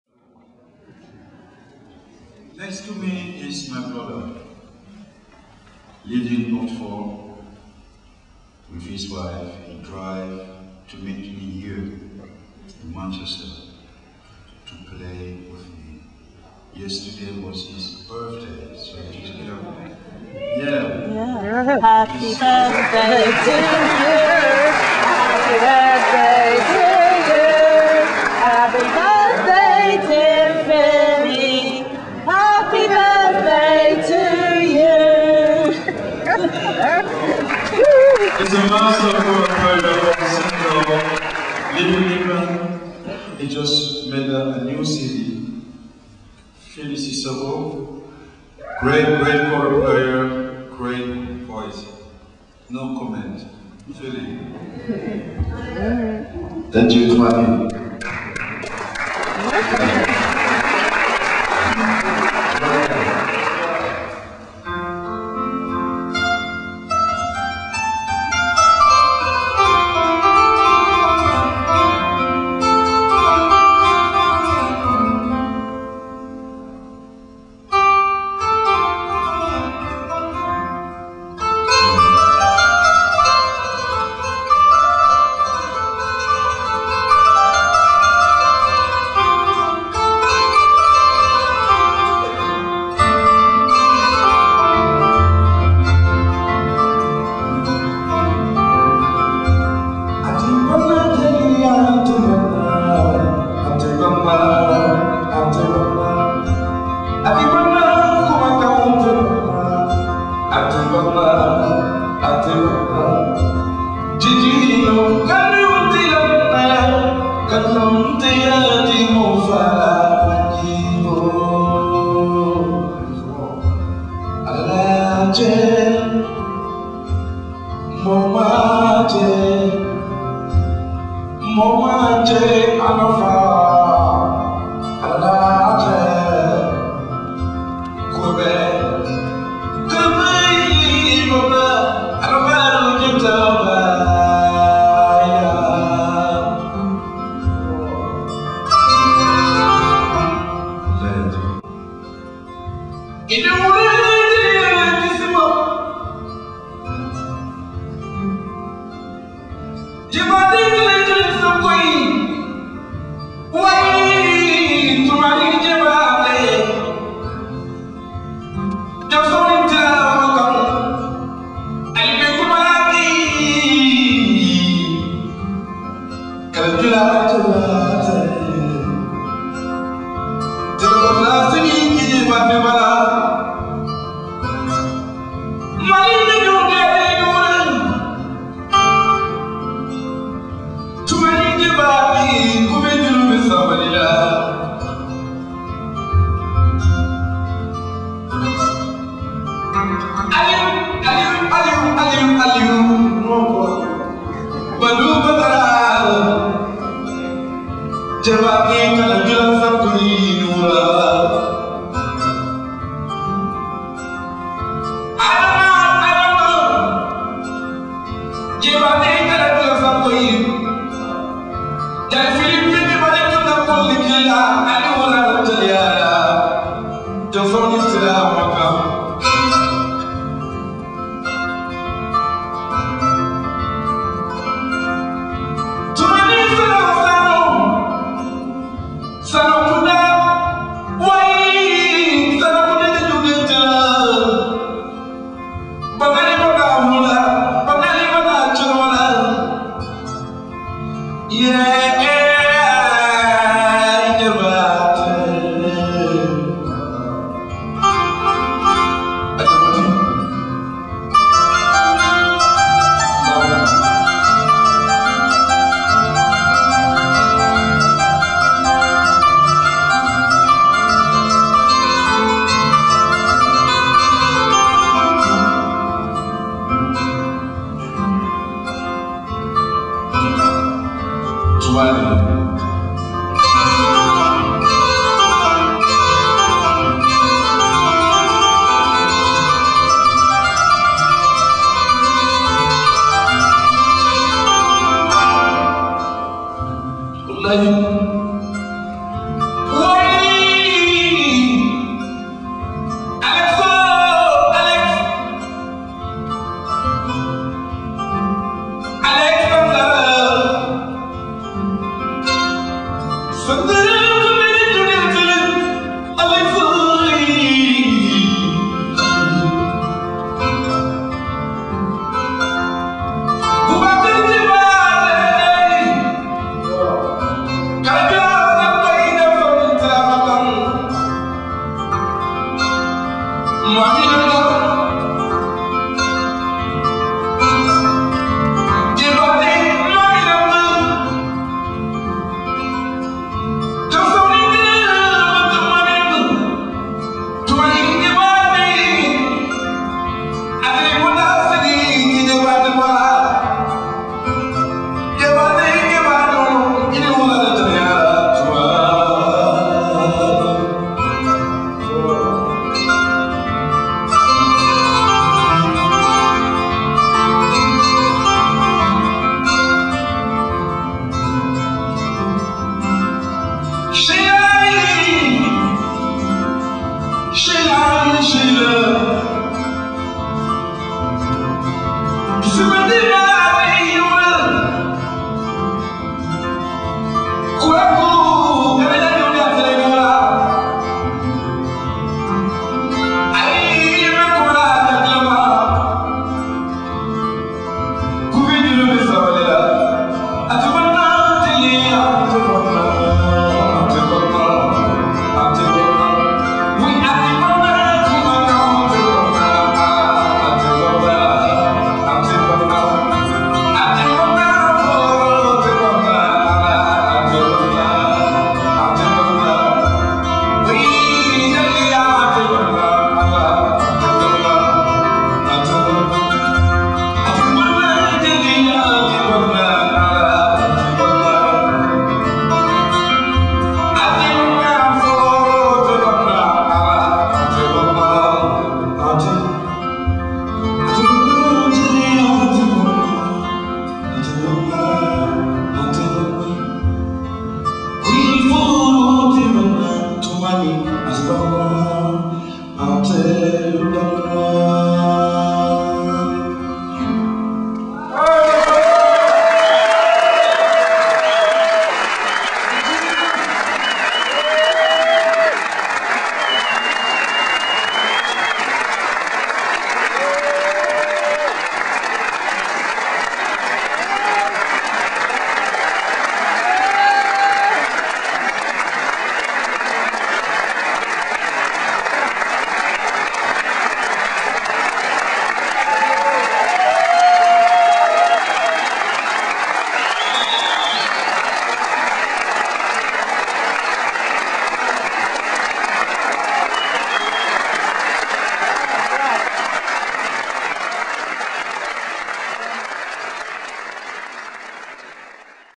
Toumani Diabate & Jali Fily Cissokho
Toumani invites Jali Fily to join him on stage at Royal Northern College of Music Manchester.
Thankyou Toumani for continuing to support us. Good luck with your tour
Fily & Christine
Film:Christine Lord KAIRA Arts 2008